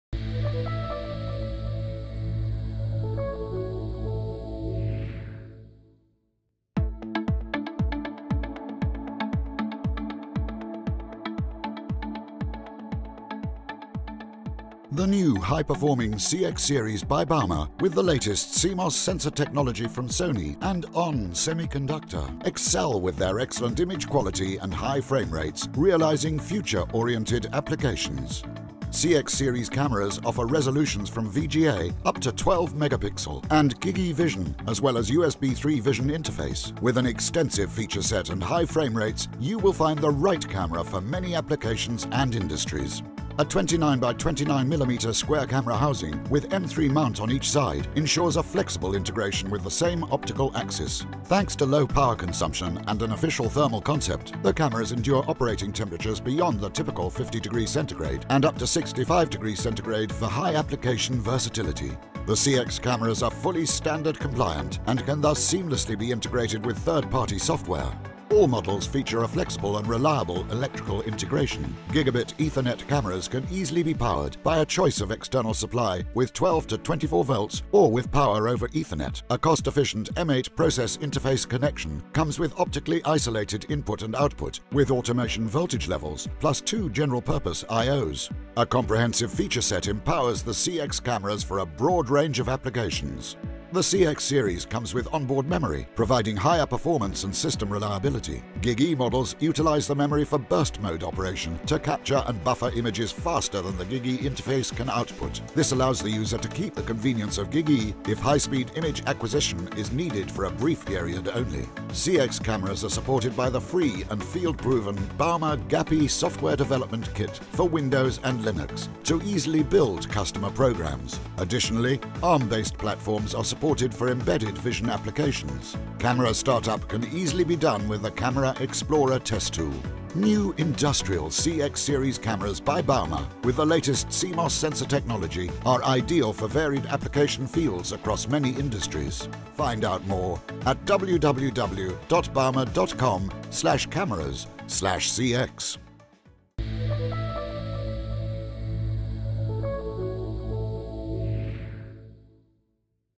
Baumer |Industrial cameras CX | Latest CMOS-Kameras
CX series GigE and USB 3.0 cameras merge high quality and reliability with the performance of the latest CMOS sensor generation Sony® Pregius™ and ON Semiconductor® PYTHON for realizing future-oriented applications.